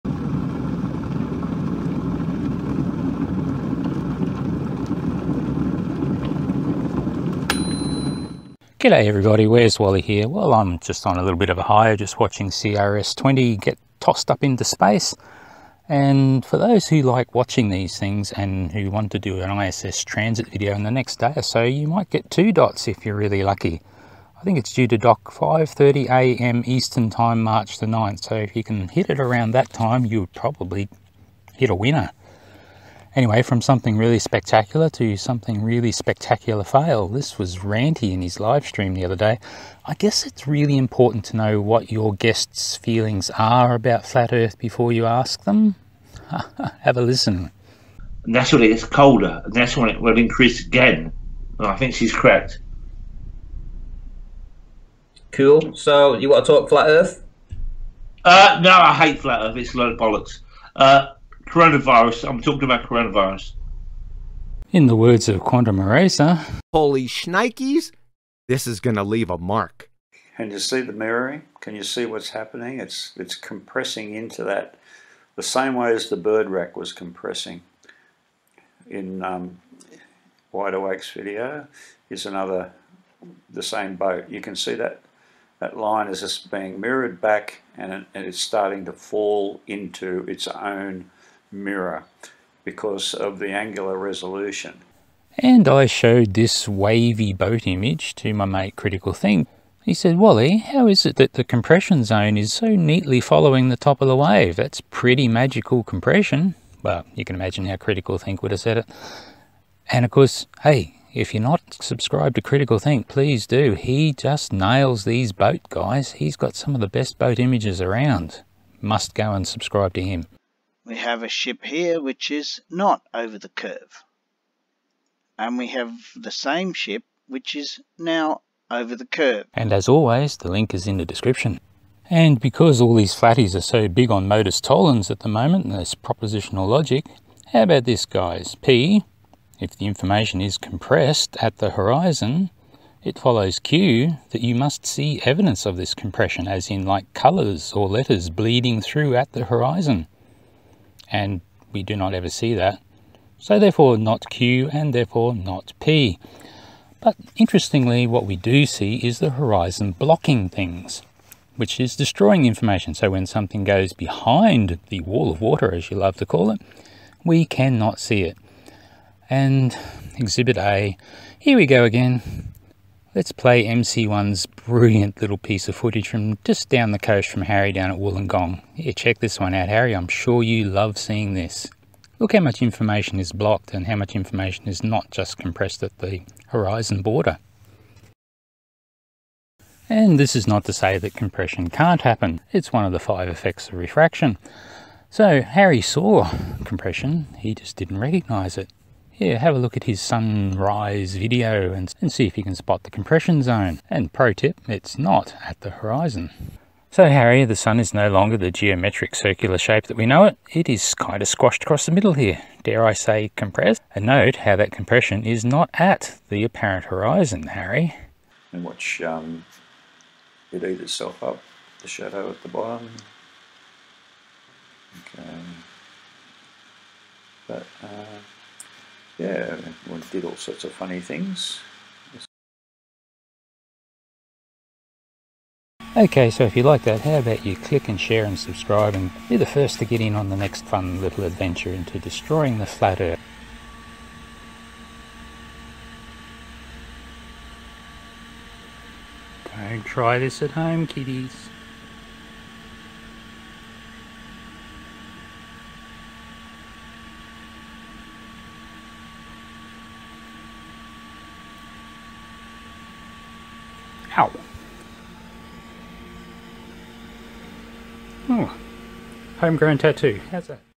Harry Sees Compression But Totally Waves It Away
Flatties like to make up ideas to explain away the real world observations that should be clearly GLOBE-SIGNS to them.

Sun rising above the horizon? Strange days indeed!